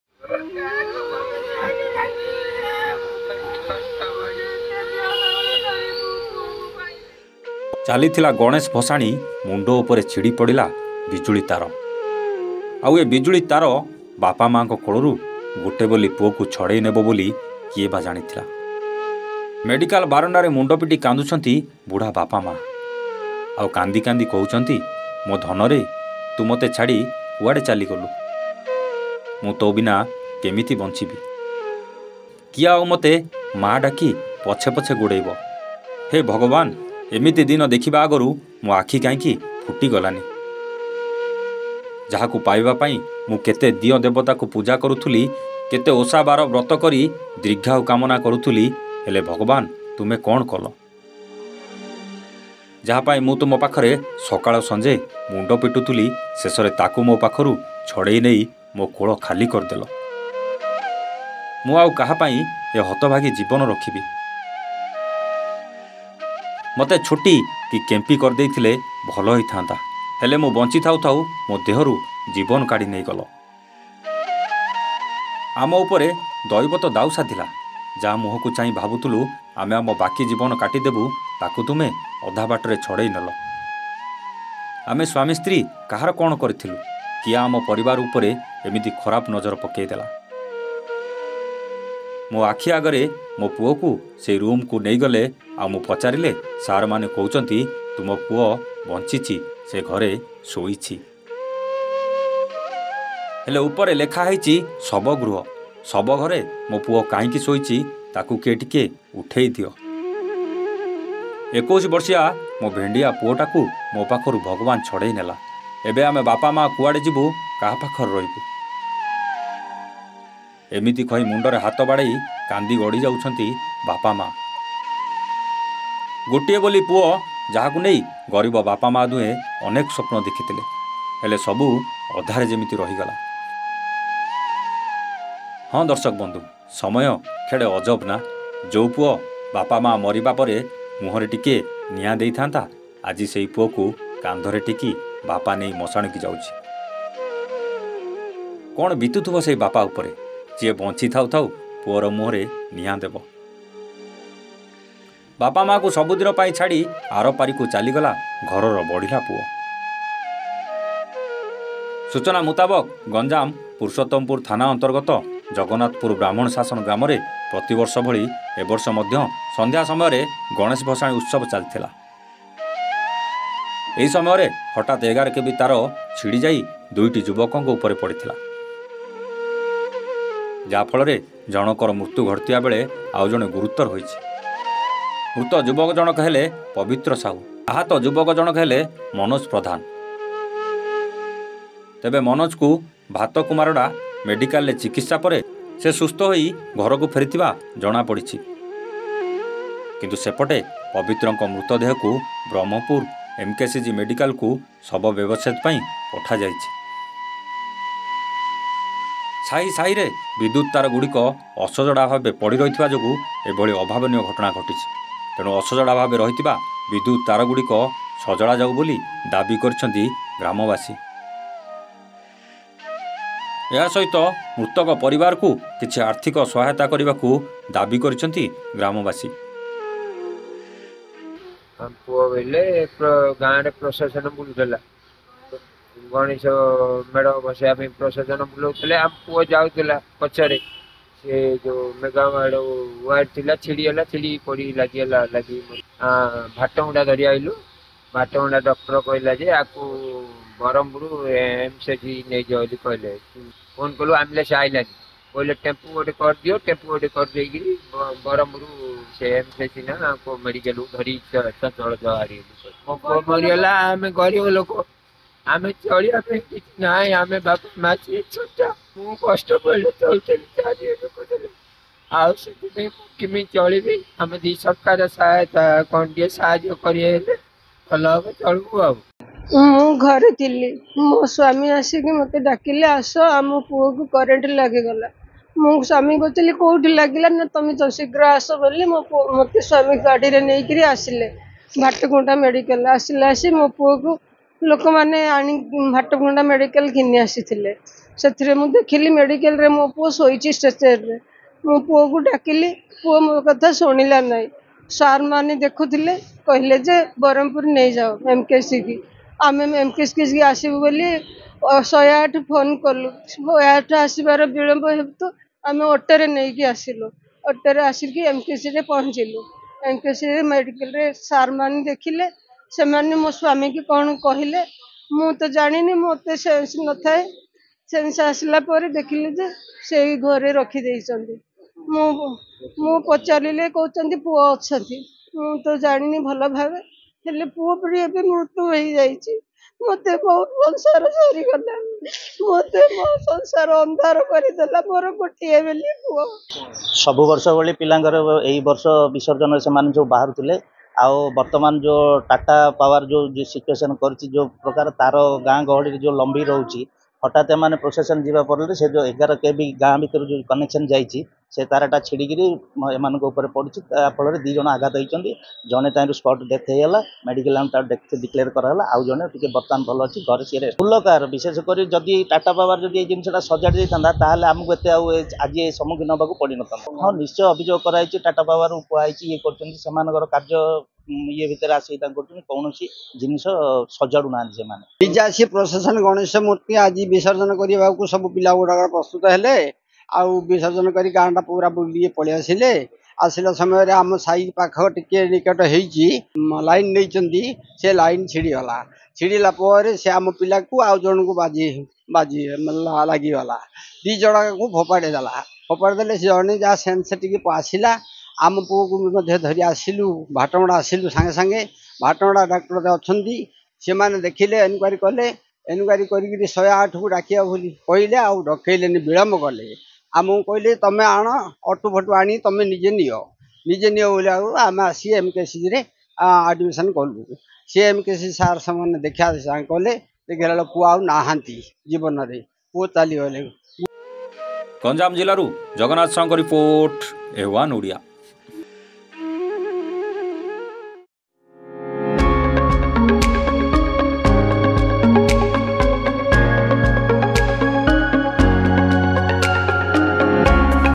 ଗଣେଶ ଭସାଣିରେ ବାପା ମାଙ୍କ ଆଖିର ଲୁହରେ ଭାସିଗଲେ ଗଜରାଜ l GANJAM NEWS l
🔆MORE UDATE🔆